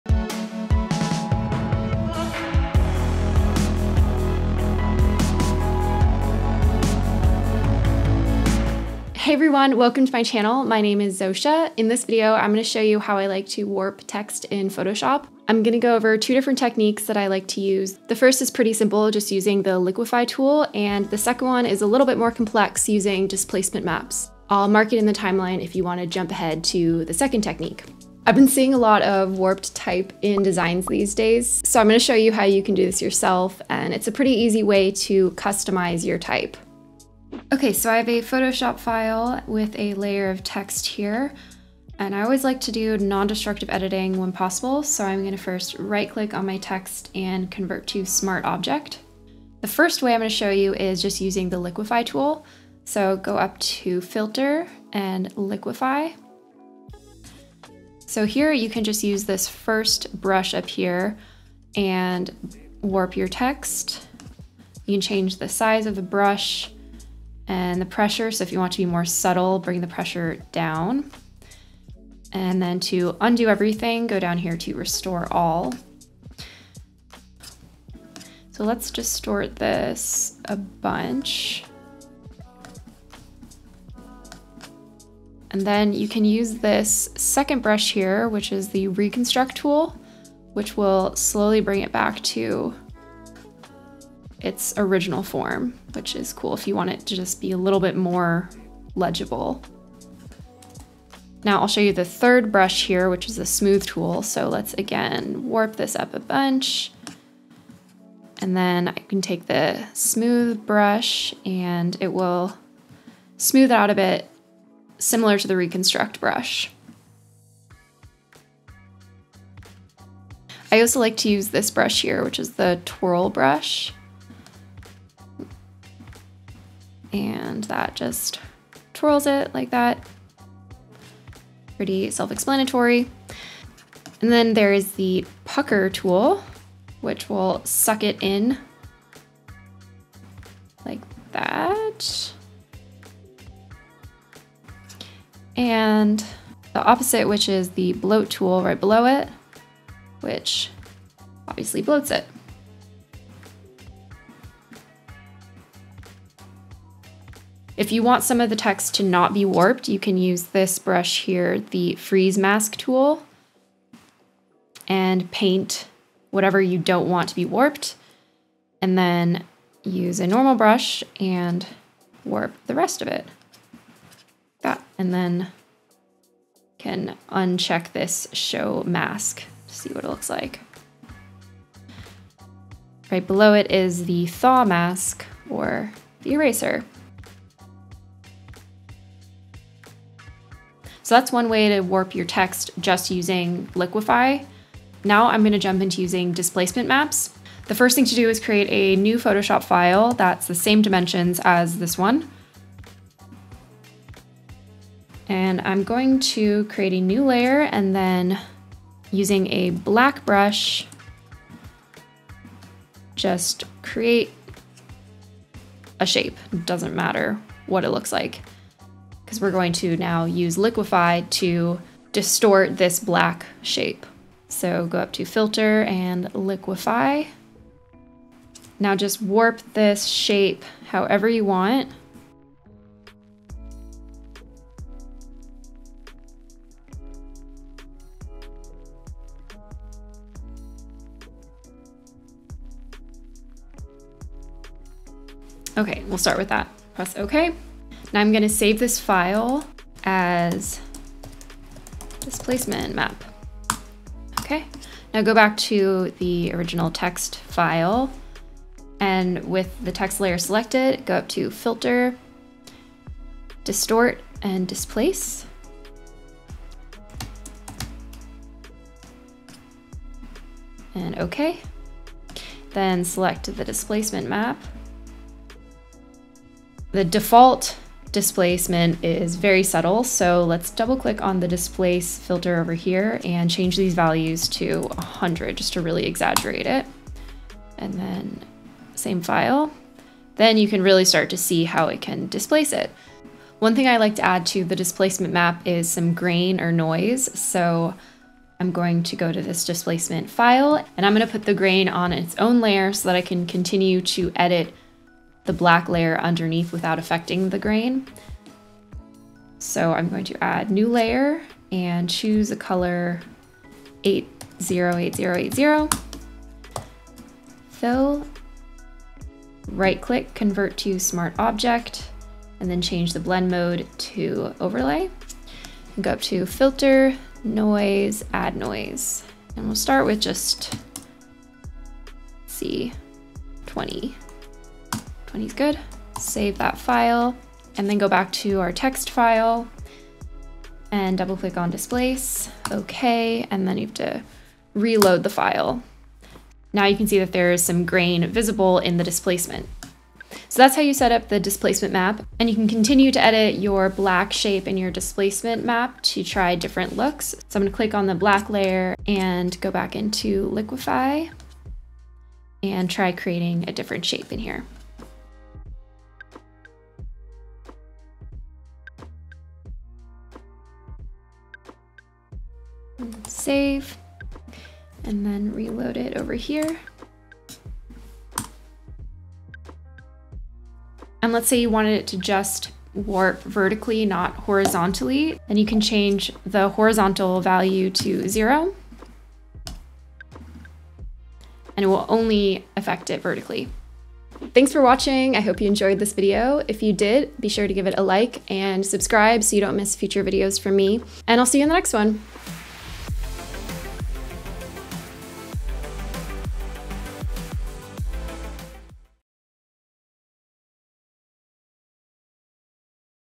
How to Warp Text in Photoshop Using Liquify + Displacement Maps
Learn to use liquify and displacement maps to distort your type in Photoshop! Let me know other techniques you'd like to learn in the comments ✦ ✦

Intro 0:00
Liquify 0:37
Displacement Map 3:15